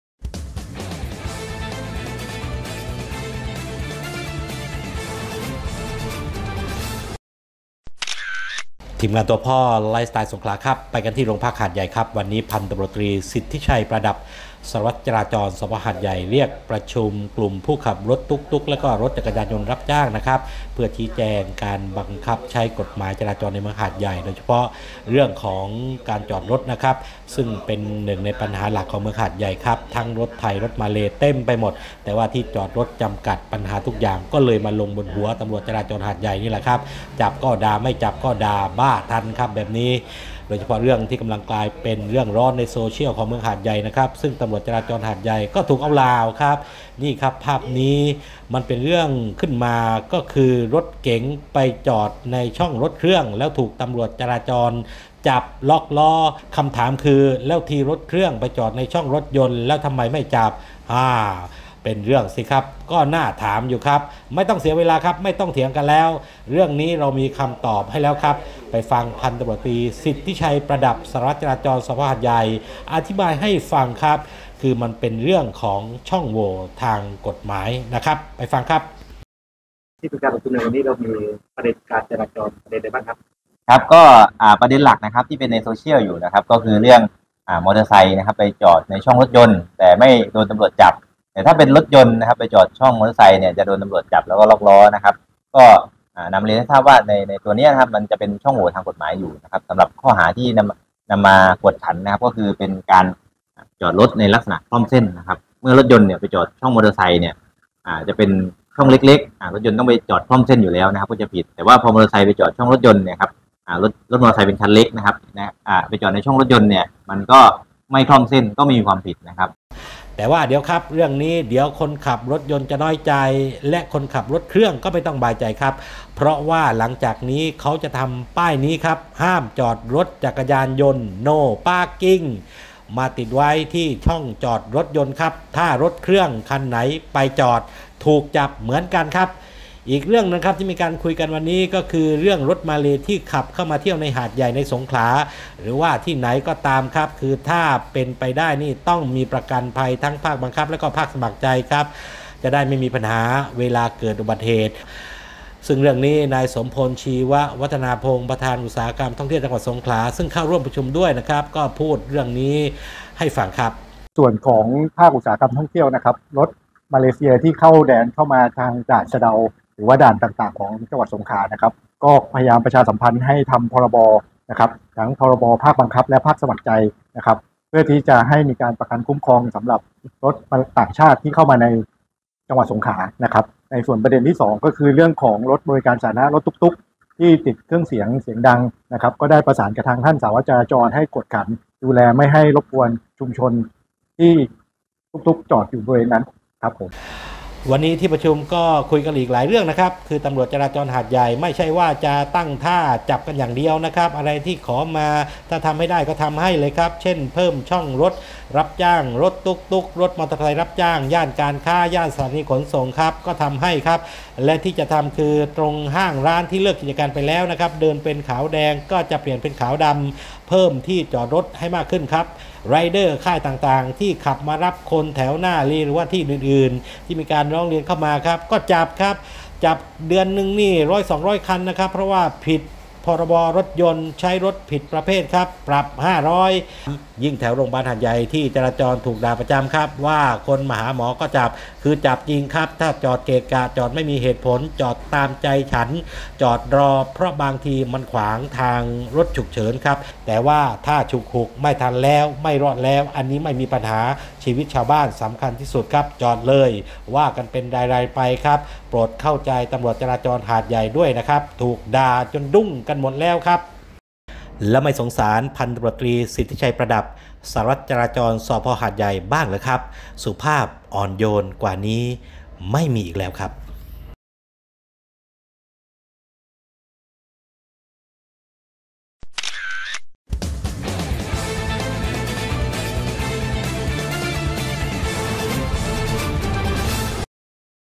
หาดใหญ่ เคลียร์ดร่าม่า!!ปัญหาจอดช่องมอไซค์
หาดใหญ่ตำรวจจราจรหาดใหญ่แก้ปัญหาดราม่าโลกแตก"รถยนต์จอดในช่องรถจักรยานยนต์ถูกจับแต่รถจักรยานยนต์จอดในช่องรถยนต์ไม่จับ"หลังเป็นประเด็นร้อนในโซเชียลเผยข้อกฏหมายถูกจับเพราะจอดคร่อมเส้น เตรียมทำป้ายห้ามจอดจยย.มาบังคับใช้
Hat Yai # Hat Yai traffic police solve the world-shattering drama "Cars parked in motorcycle lanes get caught but motorcycles parked in car lanes are not caught" after it became a hot topic on social media, revealing the law that people get caught for parking across the line. They are preparing to put up signs prohibiting motorcycle parking to enforce it.